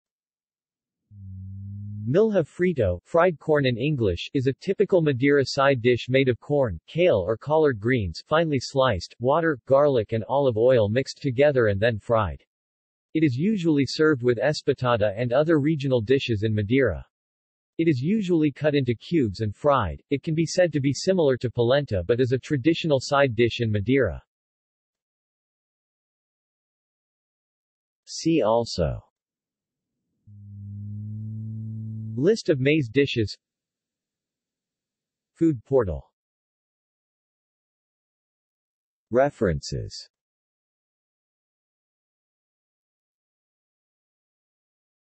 Milho Frito | Wikipedia audio article
This is an audio version of the Wikipedia Article:
Milho Frito

SUMMARY
Milho frito (fried corn in English) is a typical Madeira side dish made of corn, kale or collard greens (finely sliced), water, garlic and olive oil mixed together and then fried. It is usually served with Espetada and other regional dishes in Madeira. It is usually cut into cubes and fried, it can be said to be similar to polenta but is a traditional side dish in Madeira.